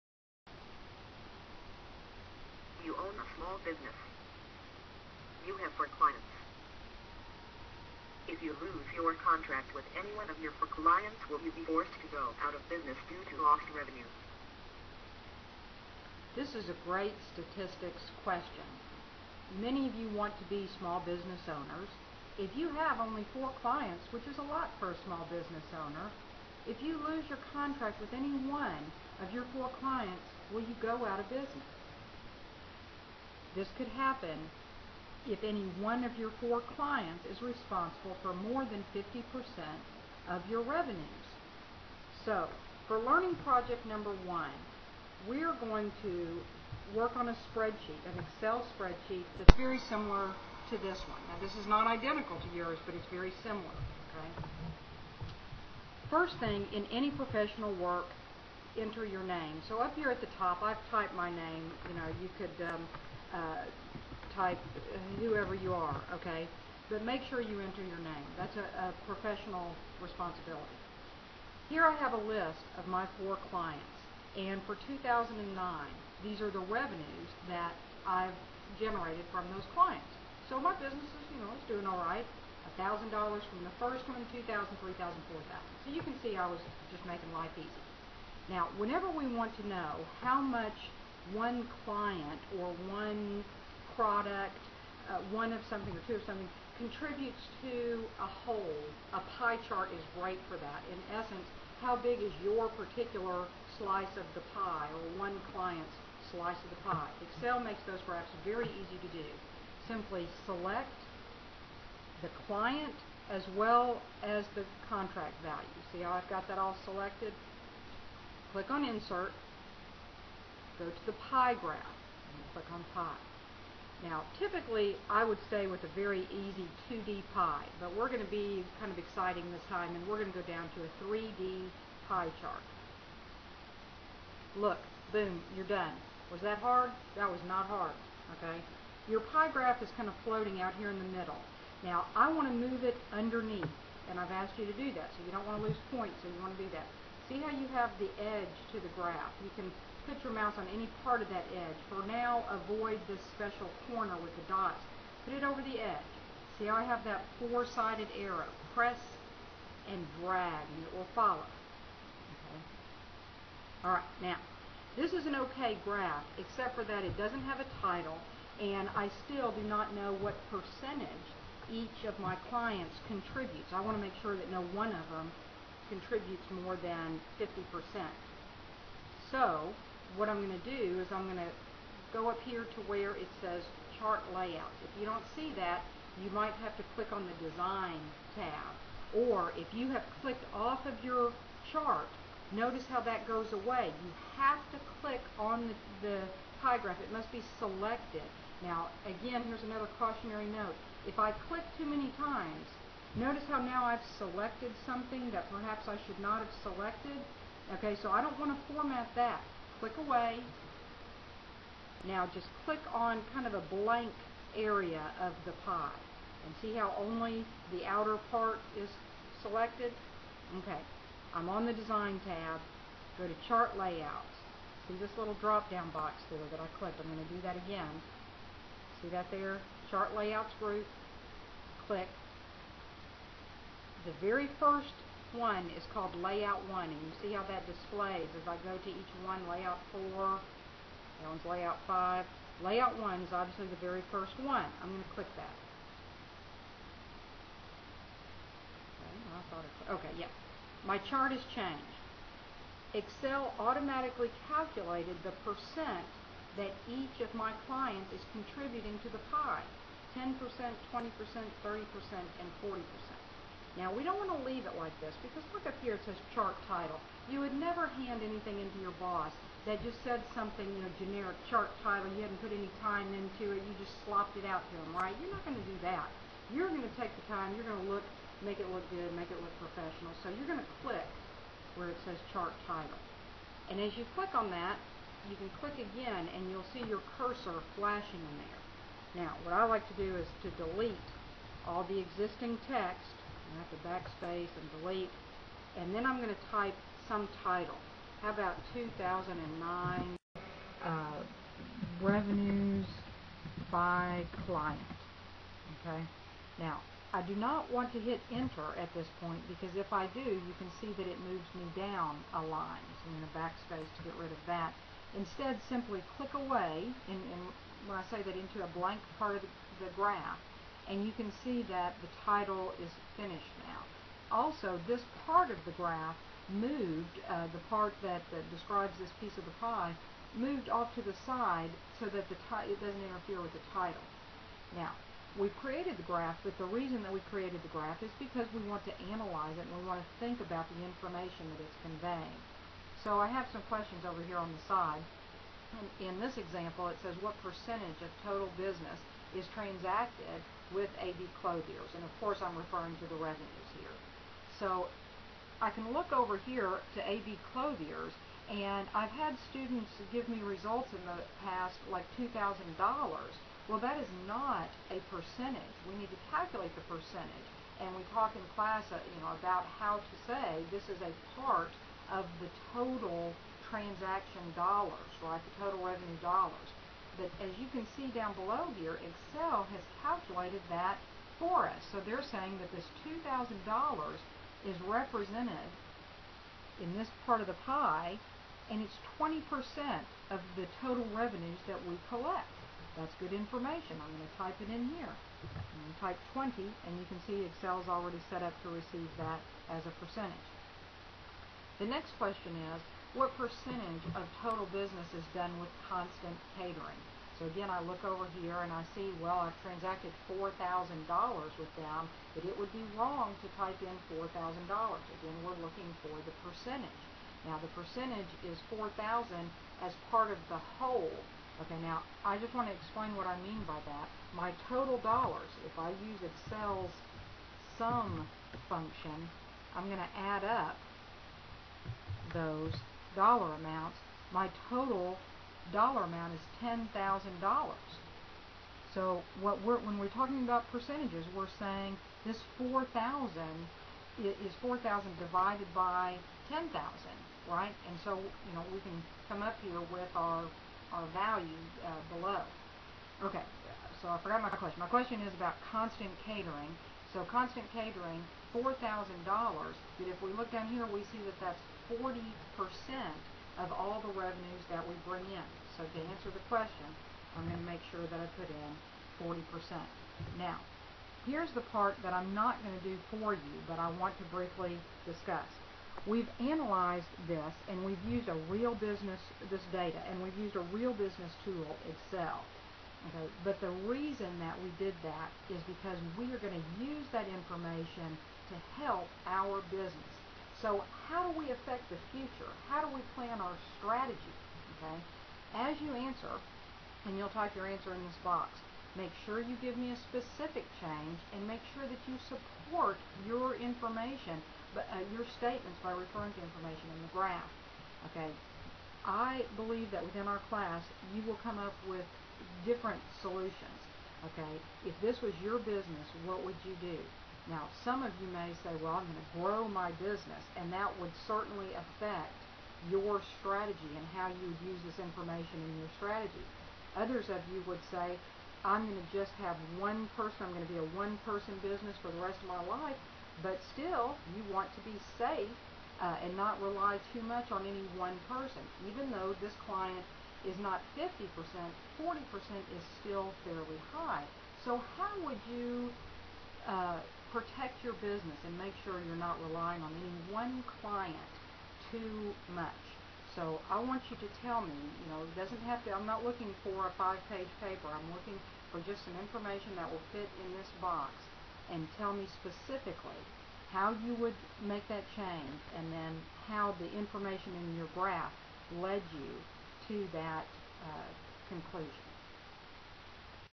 Learning Project 1 - Pie Graph
Pie graphs are a great tool for showing the contribution of a part to the whole.